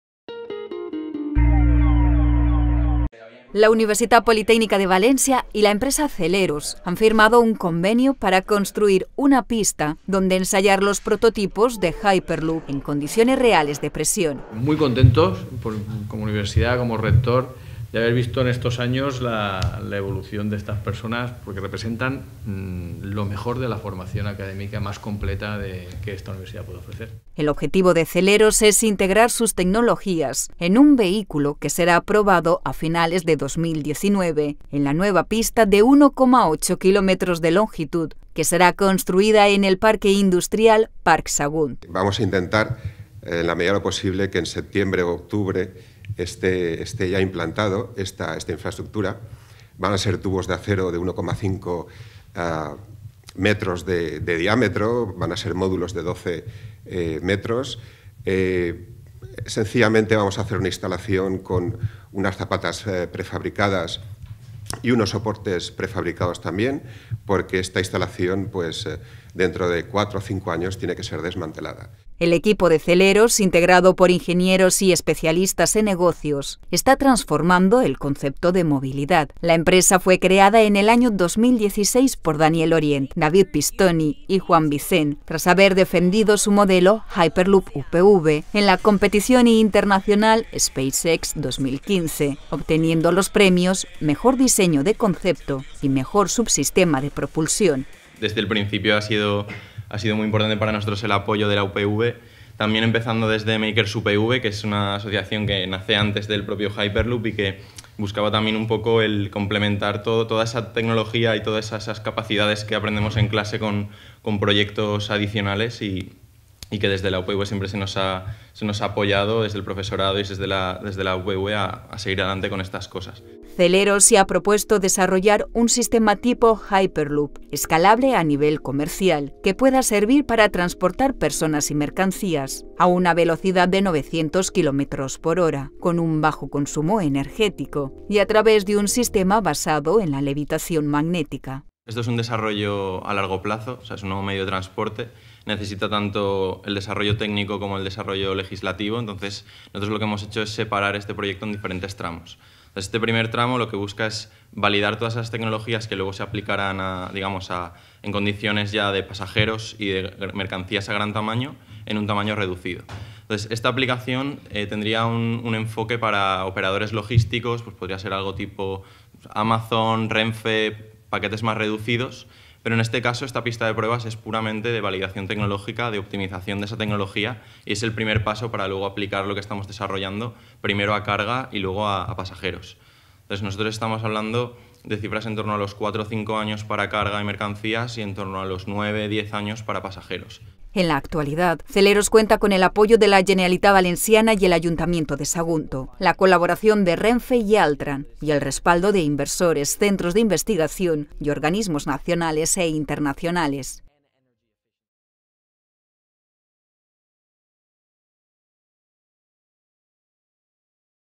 Zeleros y la UPV construirán una pista de pruebas para Hyperloop - Noticia @UPVTV, 22-02-2019
La Universitat Politècnica de València y la empresa Zeleros han firmado un convenio para construir una pista donde ensayar los prototipos de hyperloop en condiciones reales de presión.

El objetivo de Zeleros es integrar sus tecnologías en un vehículo que será probado a finales de 2019 en la nueva pista de 1,8 kilómetros de longitud que será construida en el parque industrial Parc Sagunt.

El equipo de Zeleros, integrado por ingenieros y especialistas en negocios, está transformando el concepto de movilidad.

La empresa fue creada en el año 2016 por los ingenieros Daniel Orient, David Pistoni y Juan Vicén tras haber defendido su modelo Hyperloop UPV en la competición internacional SpaceX 2015, obteniendo los premios: Mejor Diseño de Concepto y Mejor Subsistema de Propulsión.

Zeleros se ha propuesto desarrollar un sistema tipo hyperloop escalable a nivel comercial que pueda servir para transportar personas y mercancías a una velocidad de 900 Kilómetros por hora, con un bajo consumo energético, y a través de un sistema basado en la levitación magnética.

En la actualidad, Zeleros cuenta con el apoyo de la Generalidad Valenciana y el Ayuntamiento de Sagunto, la colaboración de Renfe y Altran y el respaldo de inversores, centros de investigación y organismos nacionales e internacionales.